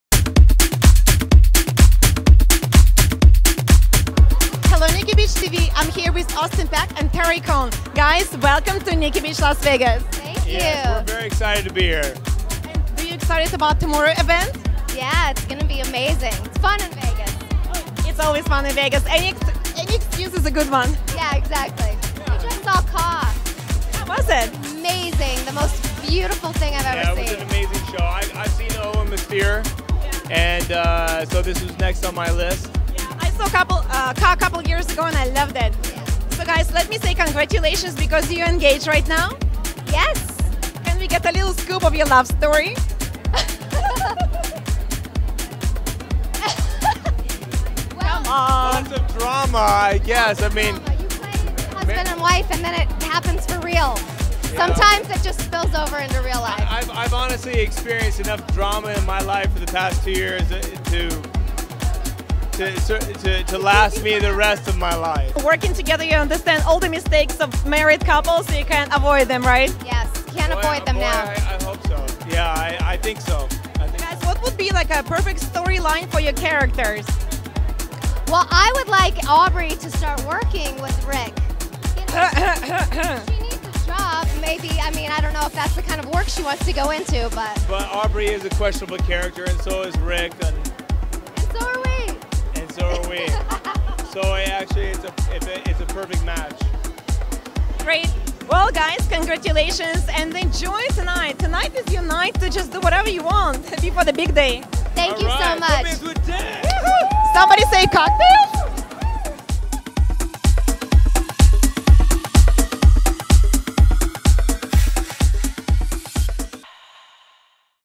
Jeniya Penrod Interviews Terri Conn and Austin Peck
Terri Conn and Austin Peck reveal what it's like to live the soap opera you portray.  Jeniya Penrod interviews this glamorous couple on the Nikki Beach Las Vegas Red Carpet.